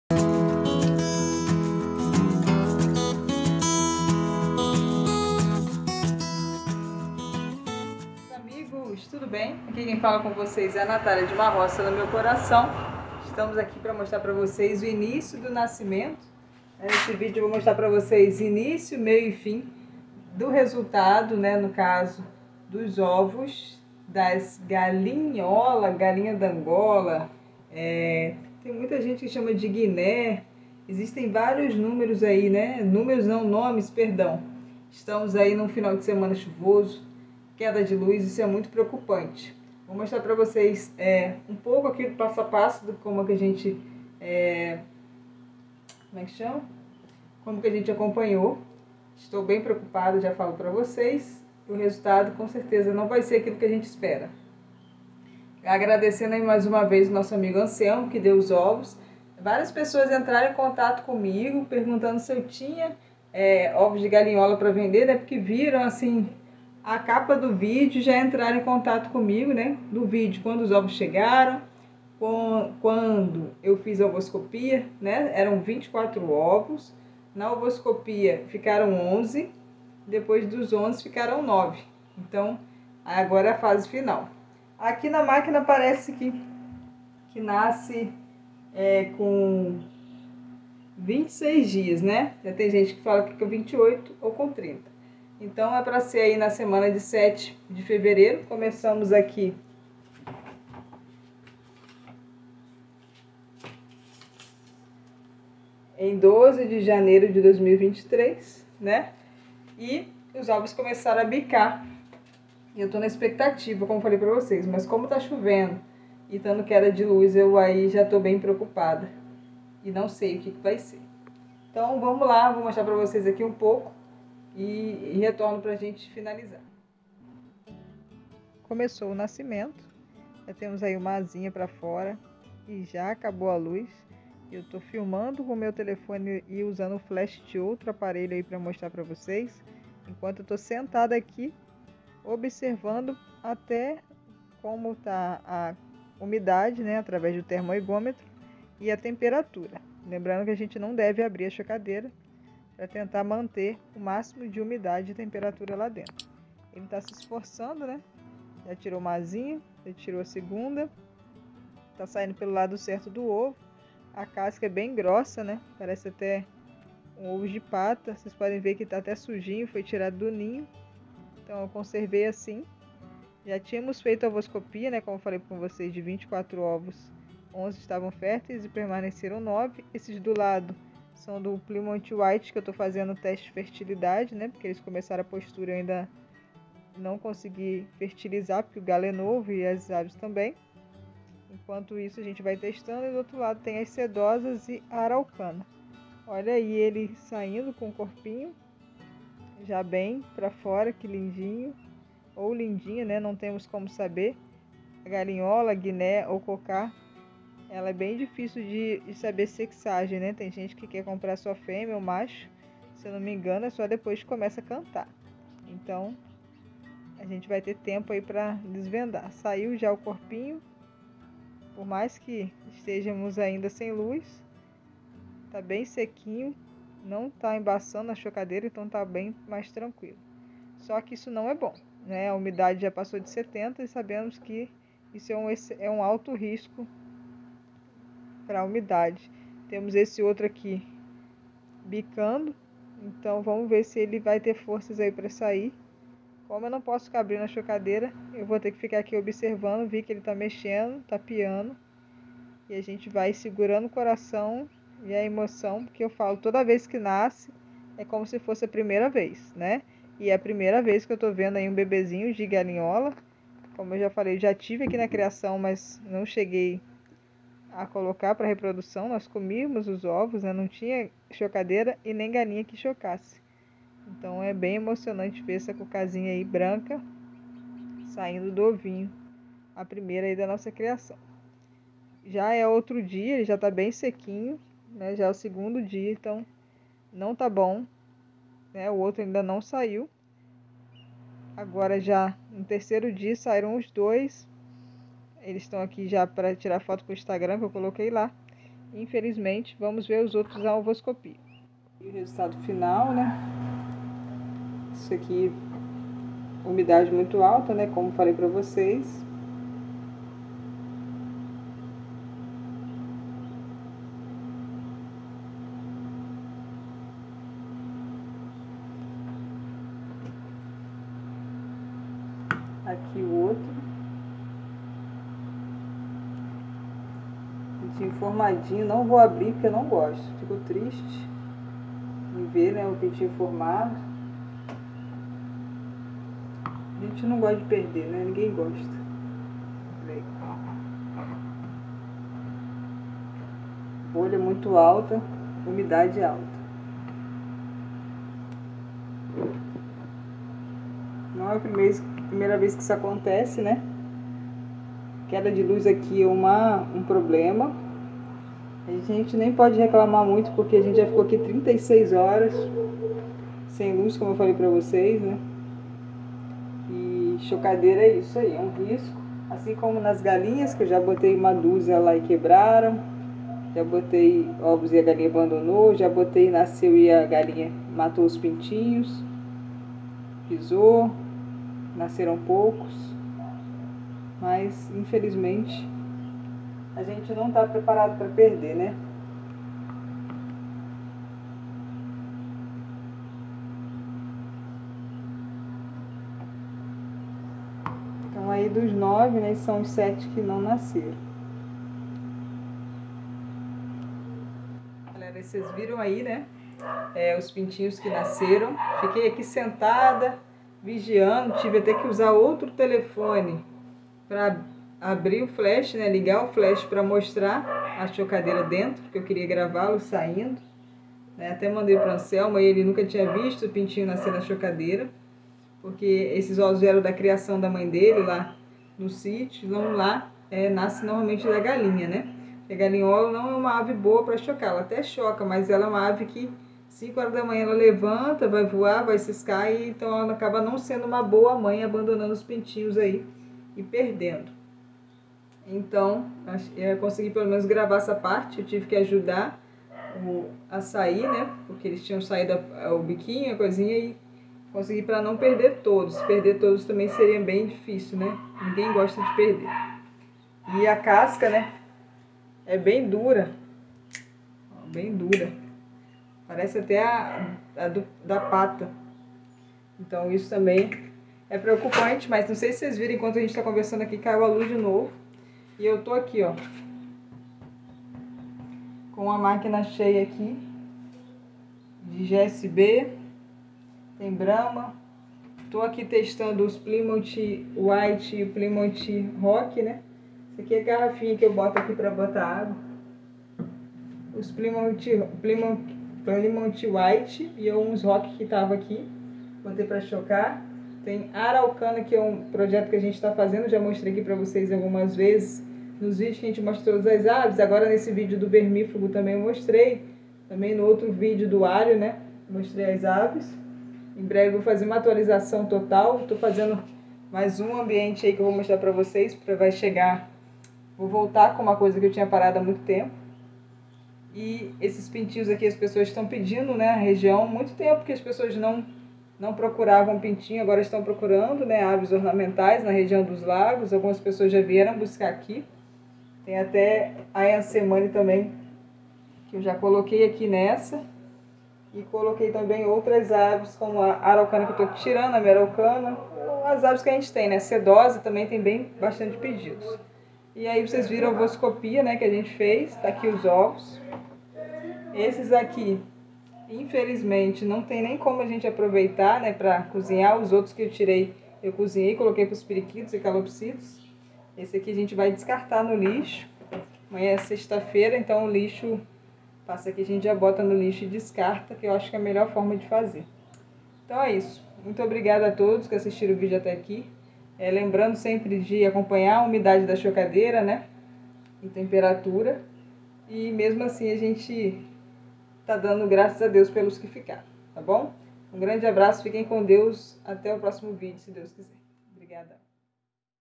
Nascimento das Angolas Brancas deu tudo errado 😓
Olá amigos recebemos ovos férteis do amigo Anselmo que mora aqui na região dos lagos,trouxe com muito carinho e iniciamos dia 12/01/23 na chocadeira,fizemos Ovoscopia e restaram 9 ovos pra nascer.
Passamos por falta de energia e infelizmente não tivemos uma boa Eclosão,mas acontece 😓